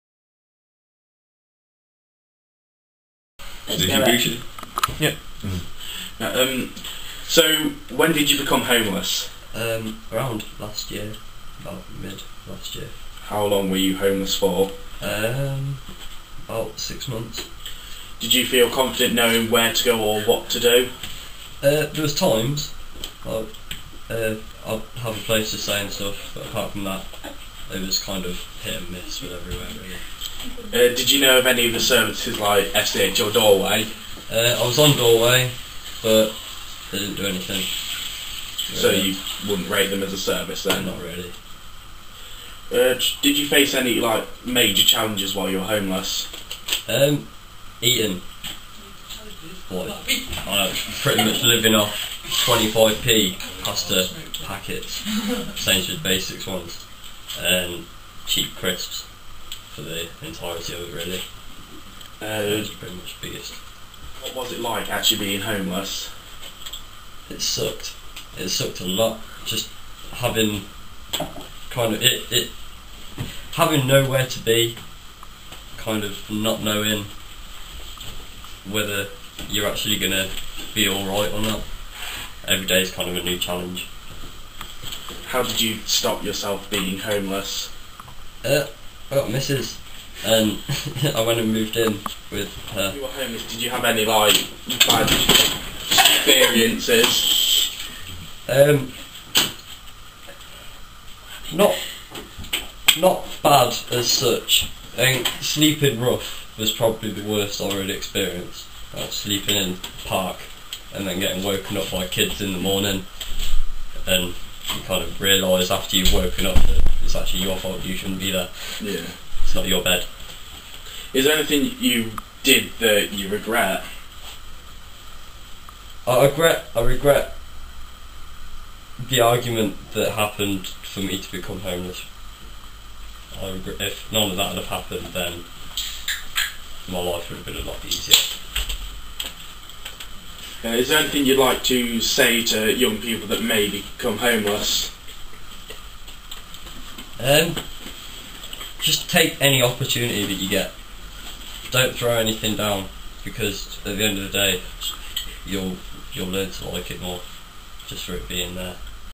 Interview with Sam Godfrey
Sam Godfrey experienced homelessness and was willing to share his experience.